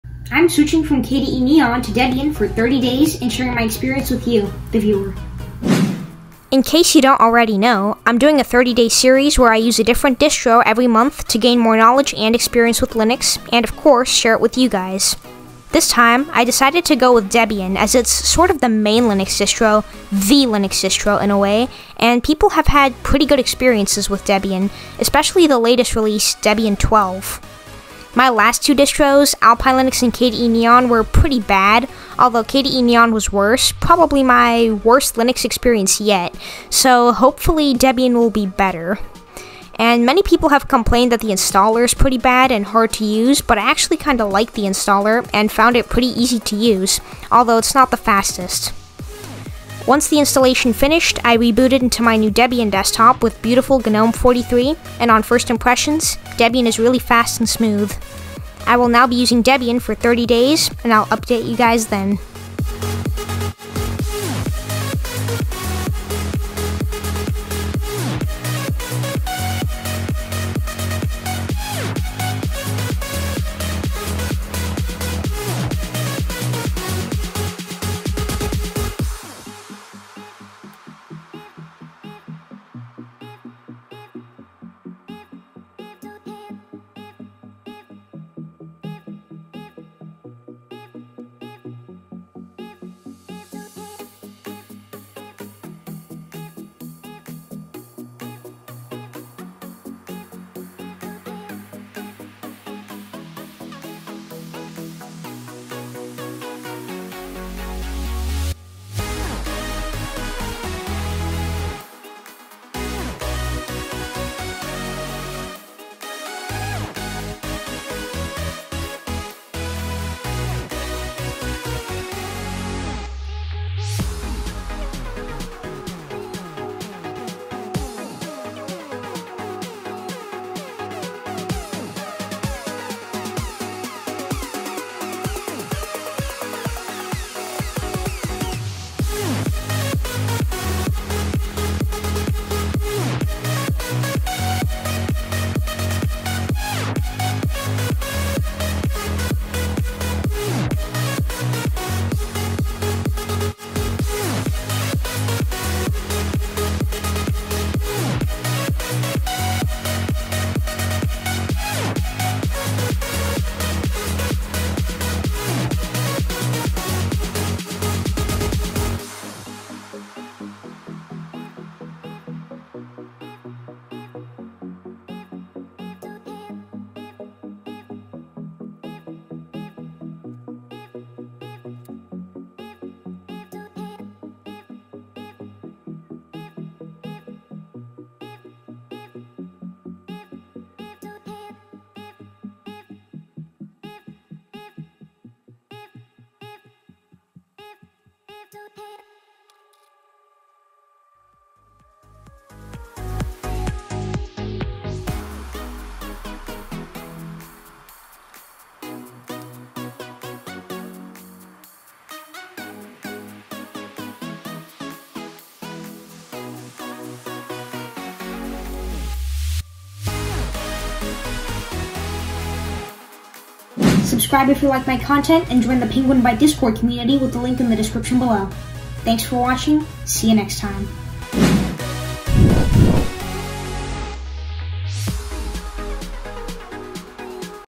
I Switched To Debian
I've used several distros recently, and unfortunately the last 2, Alpine Linux and KDE Neon, have been pretty bad experiences on the desktop. This time, I decided to try Debian as it's sort of the the "main" Linux distro in a way, and people love it, especially the latest release, Debian 12, so I'll give it a shot and come back with my experience in 30 days.